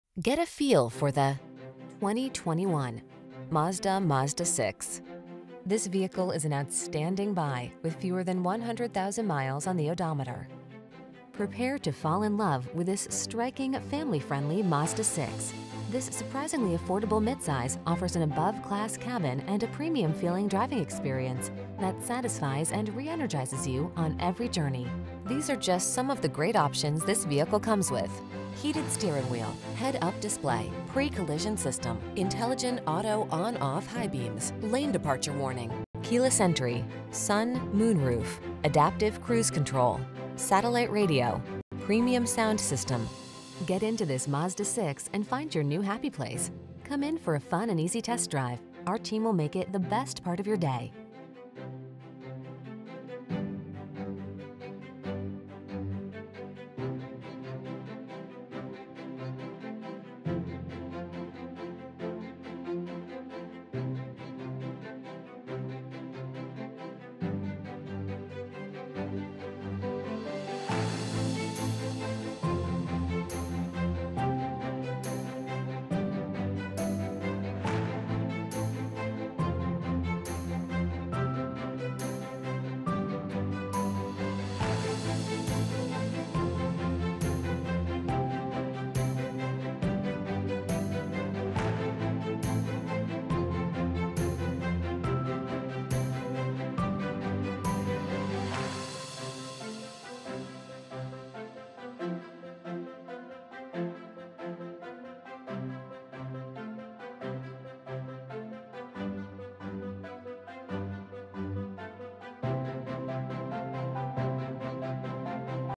2021 Mazda Mazda6 Carbon Edition OK Owasso, Tulsa, Claremore, Pryor, Broken Arrow
Pre-Owned 2021 Mazda Mazda6 Carbon Edition available at Suburban Chevrolet located in 1300 S Lynn Riggs Blvd, Claremore, OK, 74017. Servicing the Owasso, Tulsa, Claremore, Pryor, Broken Arrow area.

Get a feel for the  2021  Mazda Mazda6.  Prepare to fall in love with this striking, family-friendly Mazda 6.  This surprisingly affordable  mid-size offers an above-class cabin and a premium-feeling driving experience that satisfies and re-energizes you on every journey. These are just some of the great options this vehicle comes with: Heated Steering Wheel
, Head-Up Display, Pre-Collision System, Intelligent Auto on/off High Beams, Lane Departure Warning, Keyless Entry, Sun/Moonroof, Adaptive Cruise Control, Satellite Radio, Premium Sound System. Get into this Mazda 6 and find your new happy place. Come in for a fun and easy test drive. Our team will make it the best part of your day.